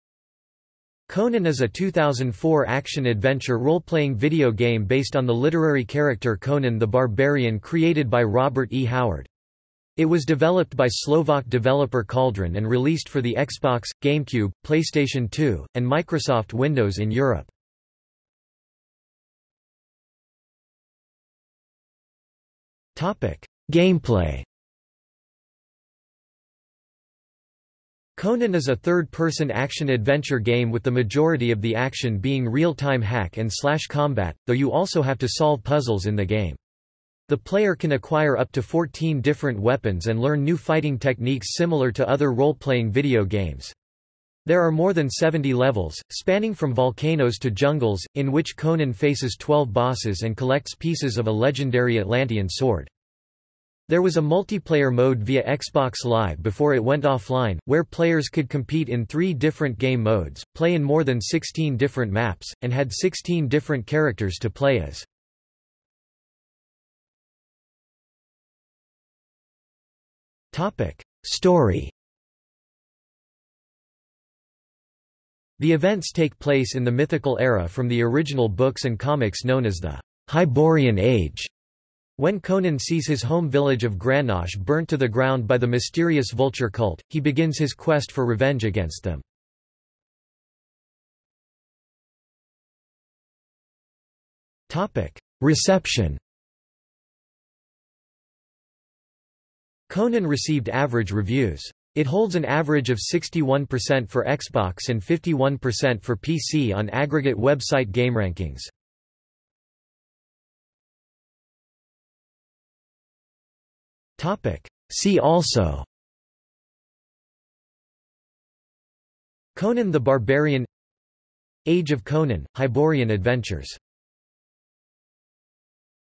Conan (2004 video game) | Wikipedia audio article
00:00:19 1 Gameplay
00:01:06 2 Story
00:01:31 3 Reception
00:01:50 4 See also

- Socrates

SUMMARY
Conan is a 2004 action-adventure role-playing video game based on the literary character Conan the Barbarian created by Robert E. Howard. It was developed by Slovak developer Cauldron and released for the Xbox, GameCube, PlayStation 2, and Microsoft Windows in Europe.